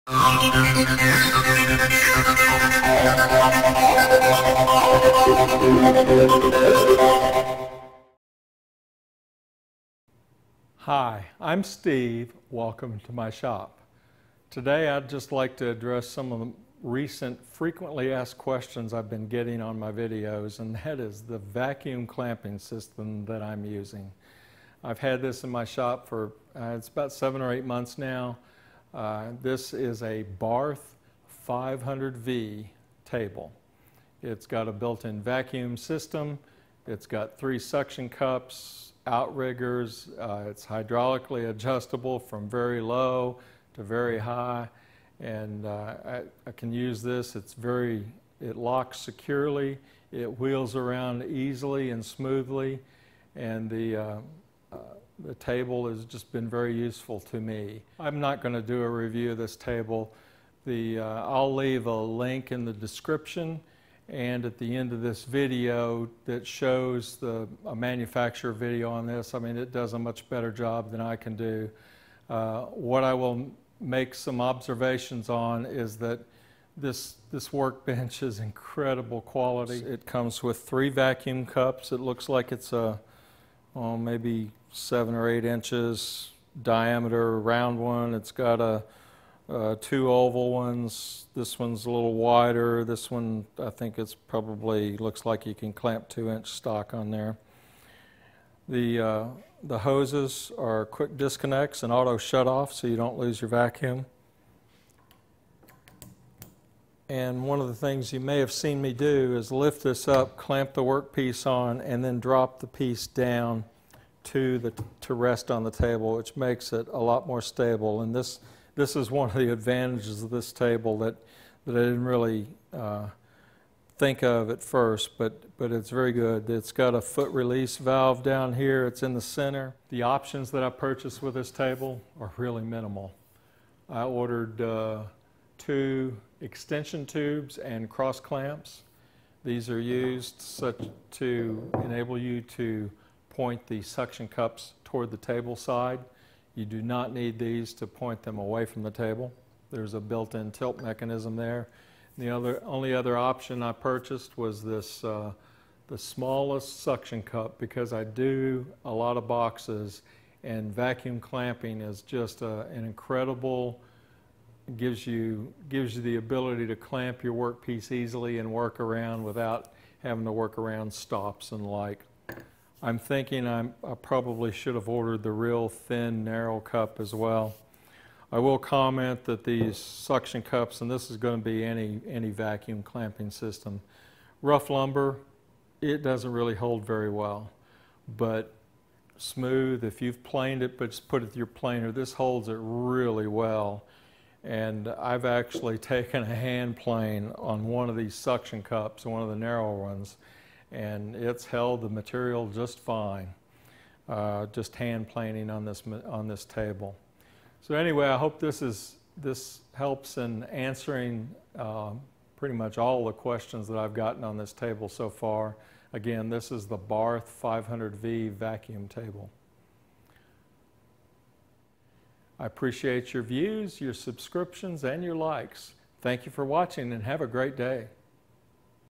Vacuum Clamping Table - Barth 500V
I have gotten a lot of questions about my vacuum clamp table used in some of my other videos.  The table is a Barth 500V.